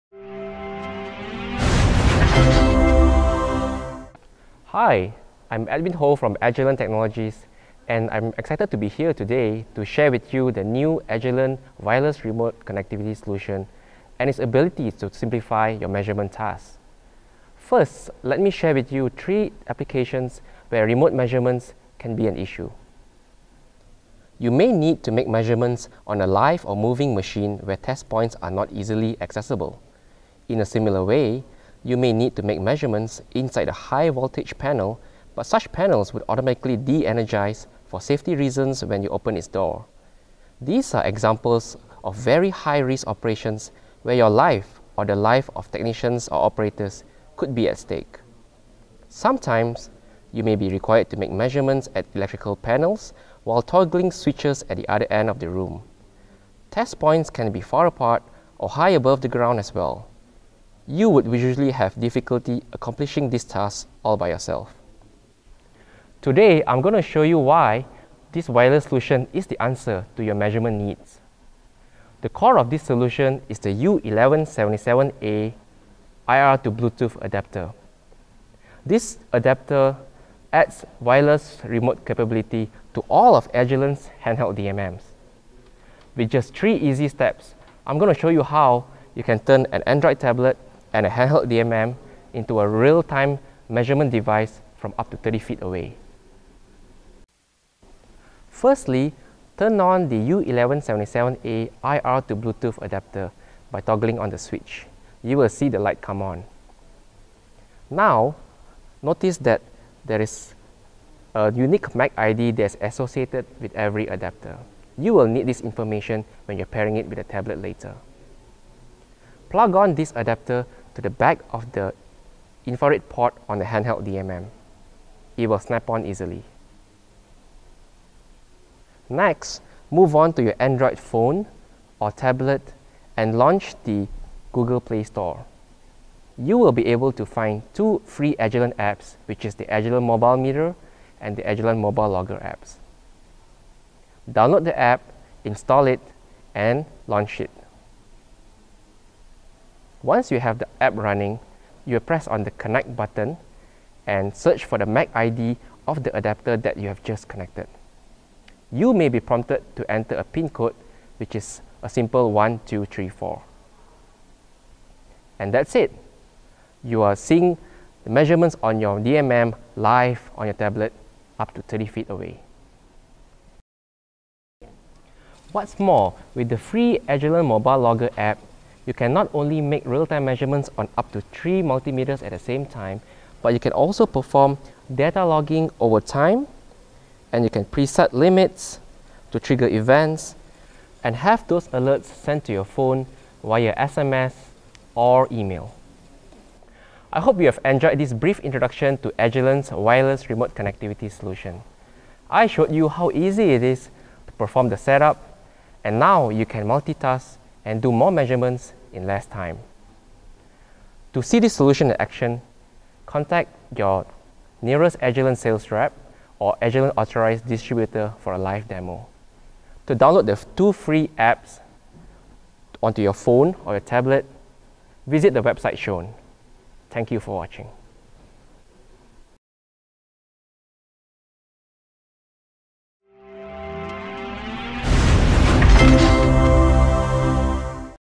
Remote Link Solution for Real-Life Work Challenges (HH DMM)
This video presents three real-life work challenges which call for a Remote Link solution for handheld digital multimeters so that measurements can be done efficiently and safely. By using an Android-based smartphone or tablet and the Keysight U1177A Infrared to Bluetooth® adapter with a Keysight handheld digital multimeter (HH DMM), you can now get your work done easily and yet safely. This video will display how you can turn your Android tablet/smartphone and your Keysight handheld digital multimeter into a real-time measurement device with just 3 easy steps. You will also learn how to use the free Keysight Mobile Meter and Keysight Mobile Logger application to enable real-time data logging of up to three handheld digital multimeters, preset limits and have the alerts sent to your smartphone via SMS or email.

Bluetooth and the Bluetooth logos are trademarks owned by Bluetooth SIG, Inc., U.S.A. and licensed to Keysight Technologies, Inc.